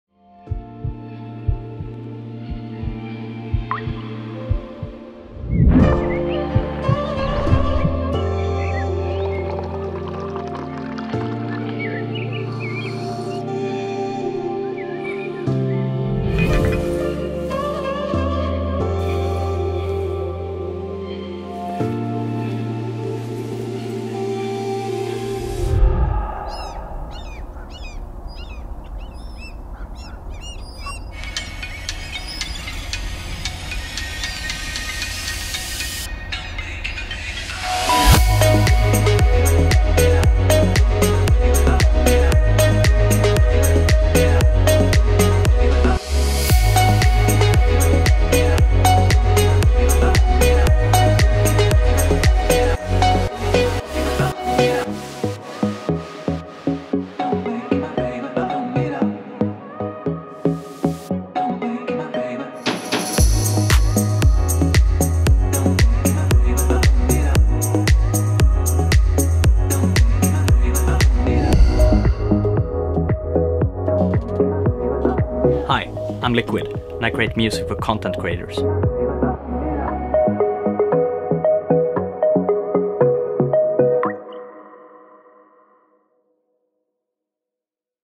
LiQWYD - A day in my life as a music producer
Hi guys!

For a long time, I've been wanting to share something personal and so I thought that a short intro to a day in my life would be nice.

So this is a normal routine in my life, waking up, drinking my coffee, and then heading off to the studio where I produce music! I am so lucky that my hobby finally turned out to be what I do full time!

Hope you will continue with me on my journey towards even greater goals, I have lots in store for 2020 and even more in the upcoming years!

The video was made by my extremely talented friend, check him out on Instagram for more of his awesome content!

Love,
Fredrik aka LiQWYD